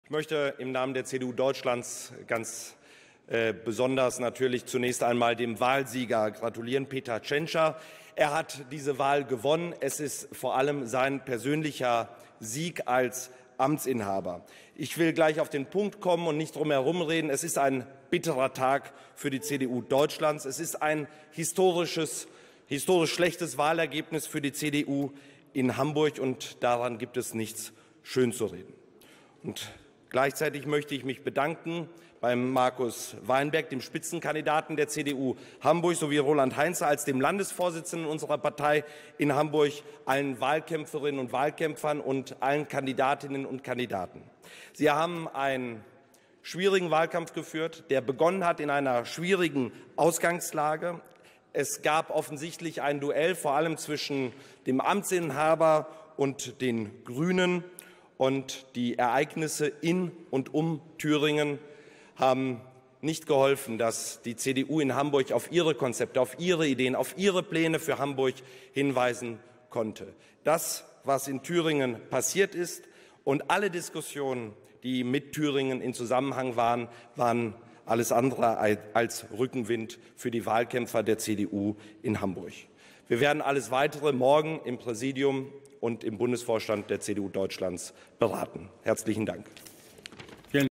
Paul Ziemiak (CDU) zur Hamburgwahl am 23.02.20
Statement von CDU-Generalsekretär Paul Ziemiak am Abend der Hamburgwahl